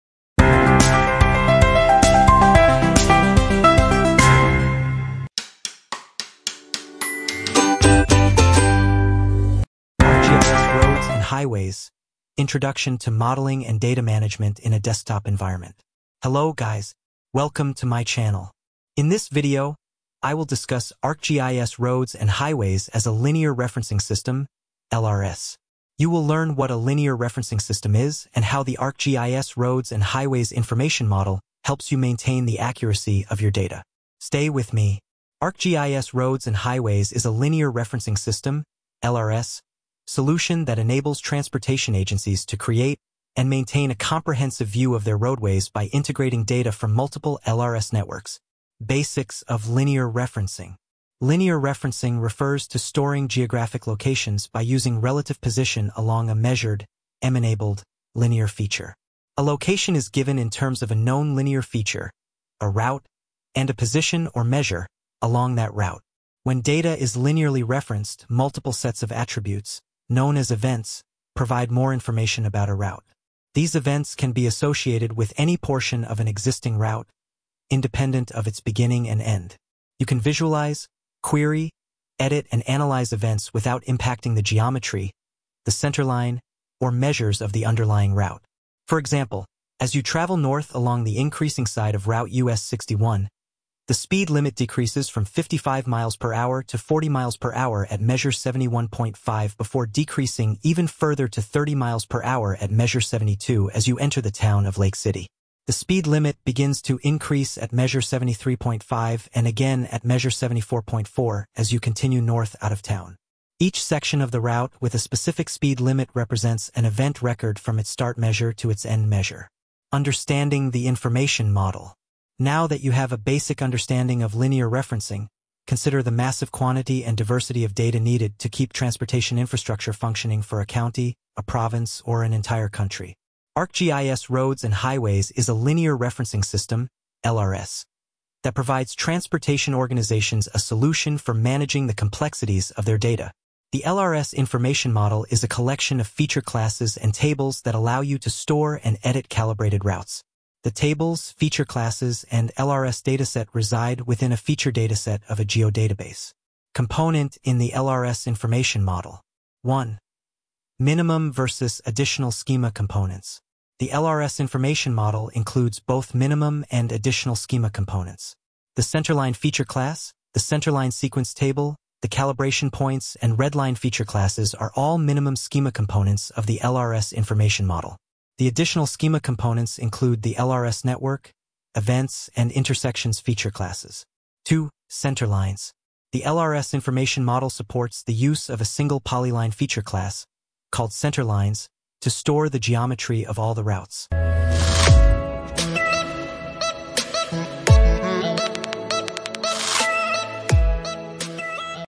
Basics of linear referencing
Linear referencing refers to storing geographic locations by using relative position along a measured (m-enabled) linear feature. A location is given in terms of a known linear feature (a route) and a position (or measure) along that route.